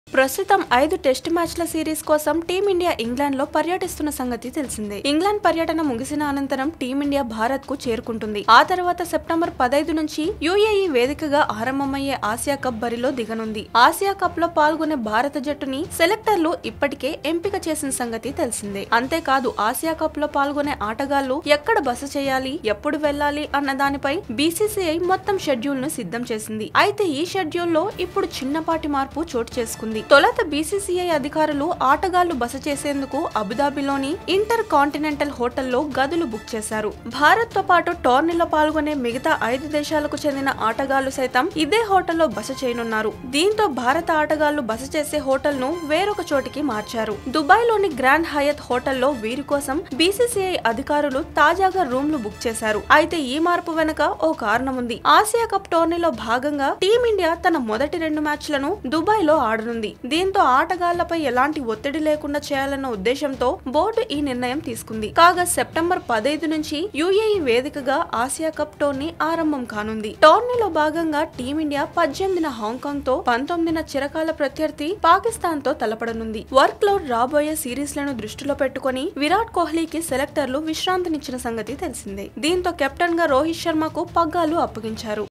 Asia Cup 2018: Bcci Changes Staying Place For Cricket Team | Oneindia Telugu
Virat Kohli has been rested for the Asia Cup as the national selectors on Saturday (September 1) announced the squad for the tournament which will be led by Rohit Sharma. The Asia Cup 2018 will be the 14th edition of the tournament and will be hosted by the United Arab Emirates from September 15 to 28. The matches will be played at the Dubai International Cricket Stadium in Dubai and Sheikh Zayed Cricket Stadium in Abu Dhabi.

ప్రస్తుతం ఐదు టెస్టు మ్యాచ్‌ల సిరిస్‌ కోసం టీమిండియా ఇంగ్లాండ్‌లో పర్యటిస్తోన్న సంగతి తెలిసిందే. ఇంగ్లాండ్ పర్యటన ముగిసిన అనంతరం టీమిండియా భారత్‌కు చేరుకుంటుంది. ఆ తర్వాత సెప్టెంబర్ 15 నుంచి యూఏఈ వేదికగా ఆరంభమయ్యే ఆసియా కప్‌ బరిలో దిగనుంది.